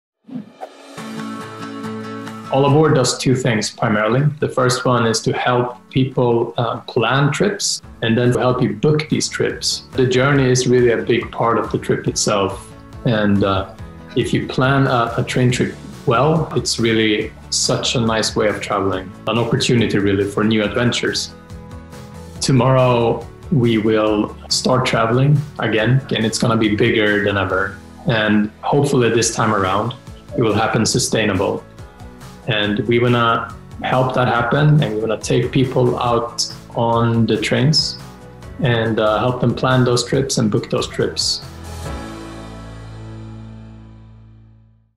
.eu Tomorrow : AllAboard.eu
Planning a train trip is an opportunity for new adventures!
All Aboard invites you to plan yours in a sustainable manner! And where is your organisation heading Tomorrow?

What are you doing tomorrow?